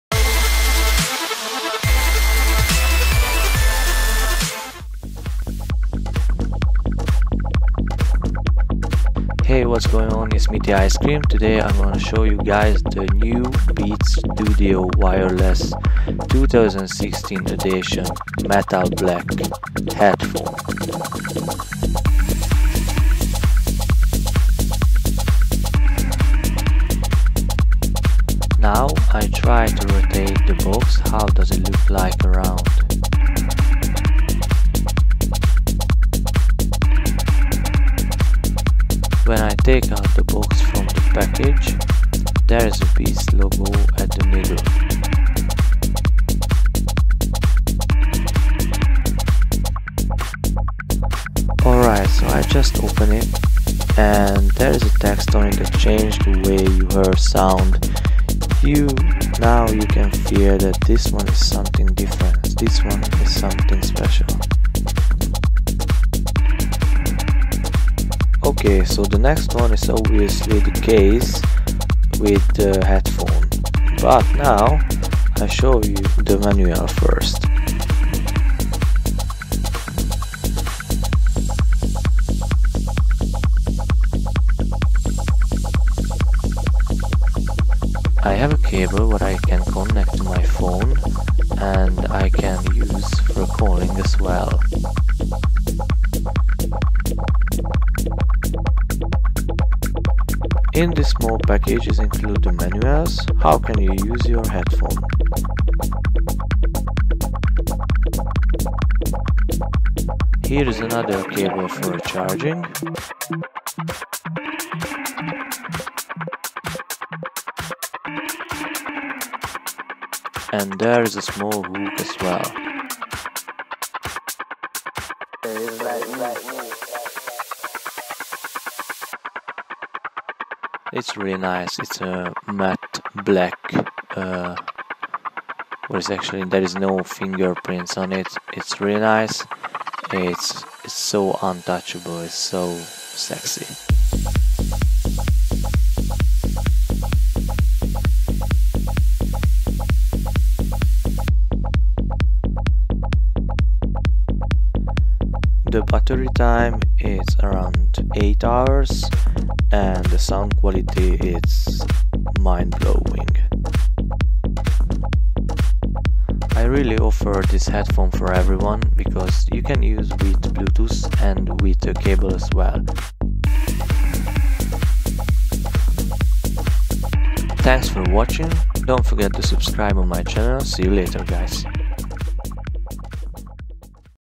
Beats Studio Wireless : Matt Black 2016 edition - Unboxing on iphone 6 (1080p60fps)
FPS in the top right corner!

My PREDATOR Laptop spec :

16GB RAM DDR4
1TB HDD
128GB SSD
17" Full HD
NVIDIA GeForce GTX 980M 4gb GDDR5